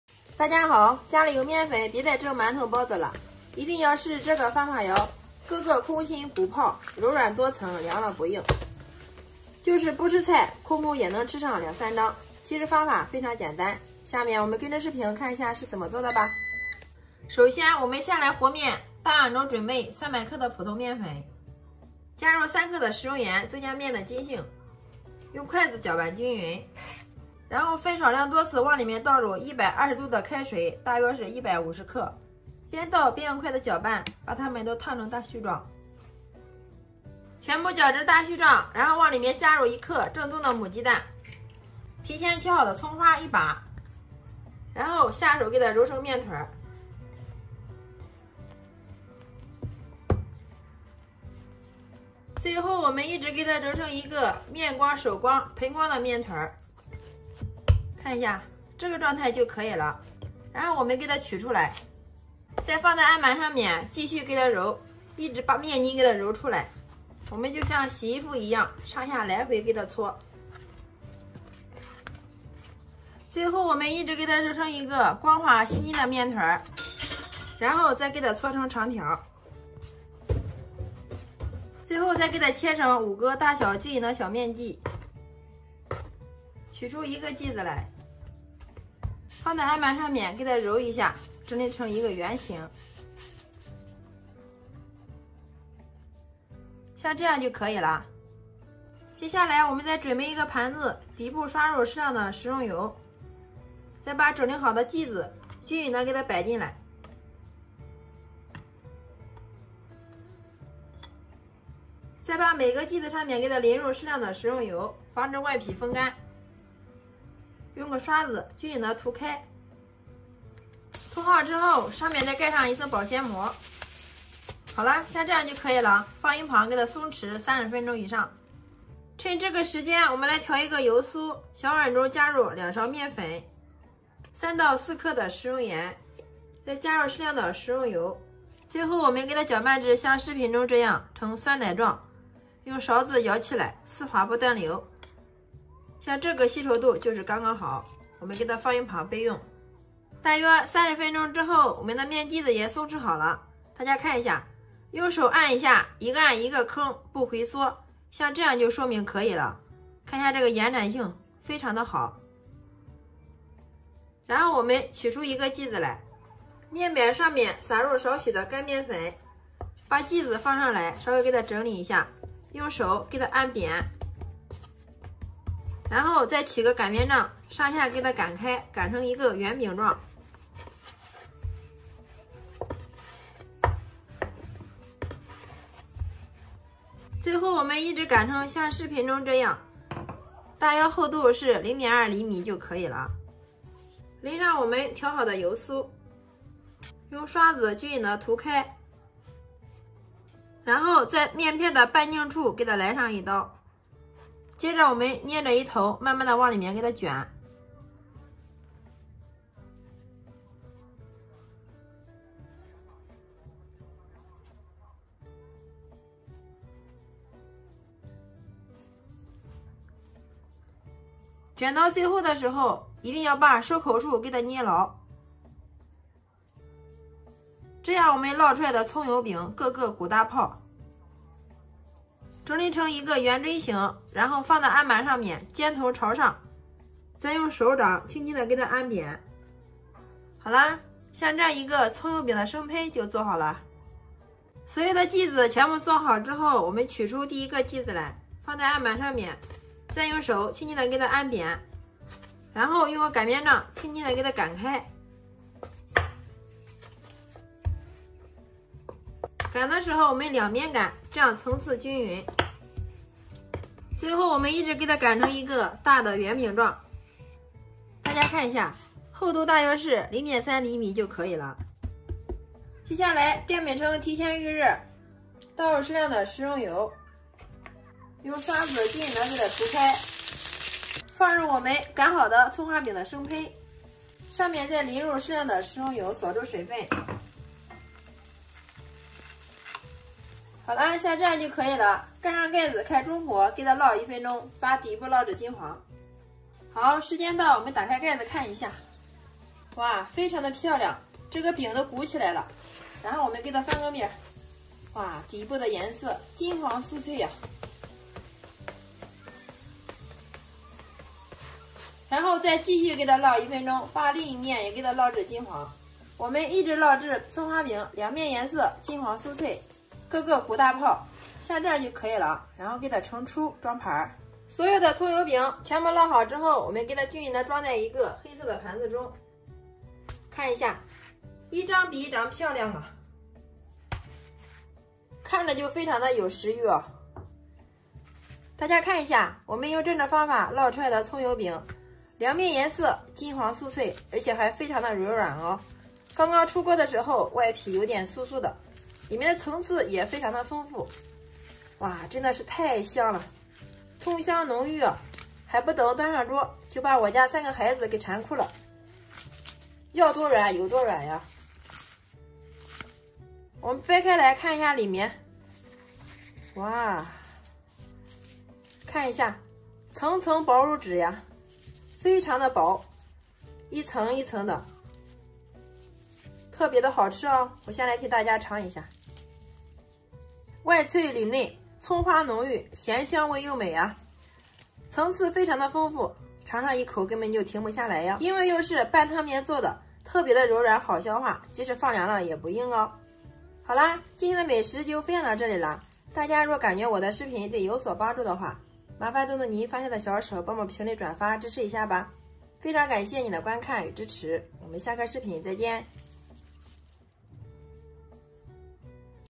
面粉不蒸包子馒头了，我家10顿有9顿都这样做，个个鼓泡层次多【刘二姐美食】
面粉不蒸包子馒头了，我家10顿有9顿都这样做，个个鼓泡层次多

刘二姐美食 美食教程 面食 "在视频制作中，我们将动物福祉作为首要考虑，所有动物参与者都受到了妥善的照顾。"